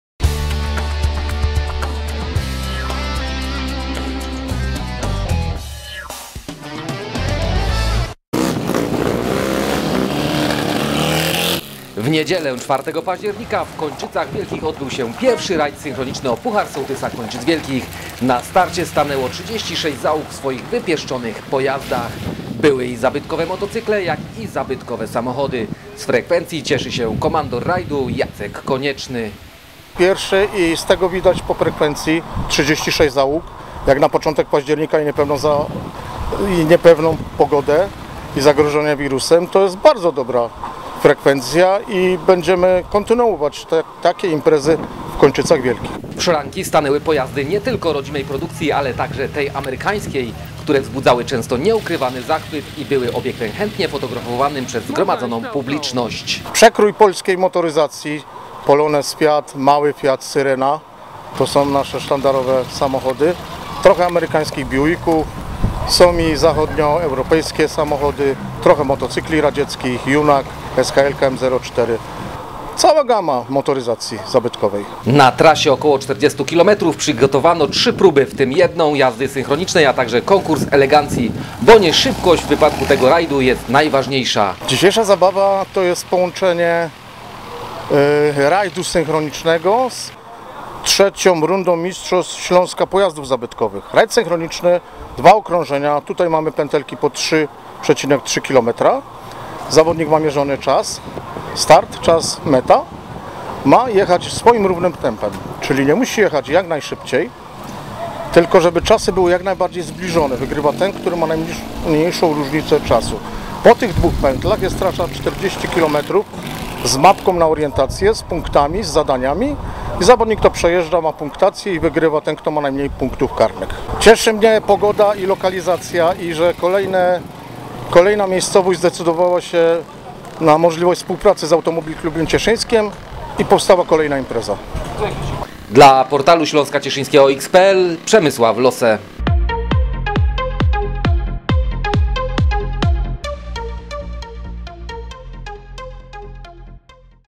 I Rajd Synchroniczny o Puchar Sołtysa Kończyc Wielkich
W niedzielę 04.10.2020 w Kończycach Wielkich odbył się I Rajd Synchroniczny o Puchar Sołtysa Kończyc Wielkich. Na starcie stanęło 36 załóg w swoich wypieszczonych pojazdach. Były i zabytkowe motocykle jak i zabytkowe samochody. Zapraszamy do materiału wideo z tego wydarzenia.